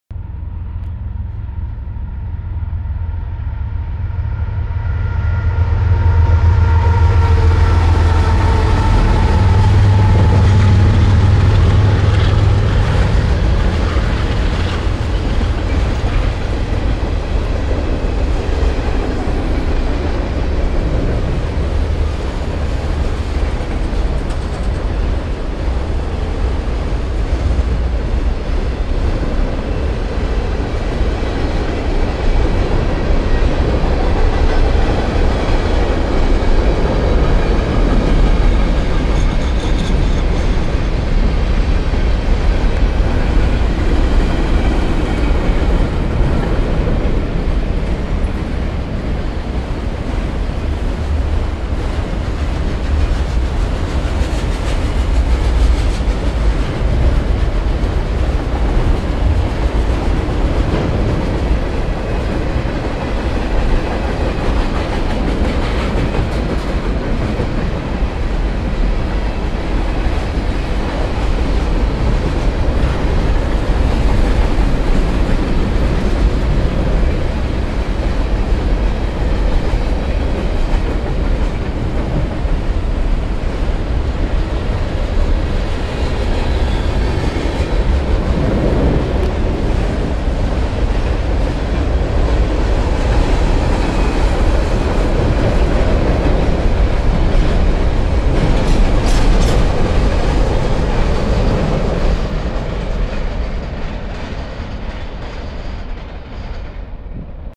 (Double BNSF + NS!!) CPKC 134 East in Galt, ON
While walking to school, this 134 came out of nowhere making me go to an interesting "emergency" spot to record it leading to an interesting angle.

Symbol: 134
Units: BNSF 3976, BNSF 7615, NS 7633
Origin: Oak Yard, Detroit MI
Destination: Agincourt Yard, Toronto ON
Subdivision: Galt
City: Cambridge
Location: south of Elgin/Clyde intersection
Date Taken: February 9th 2024 at 7:51 AM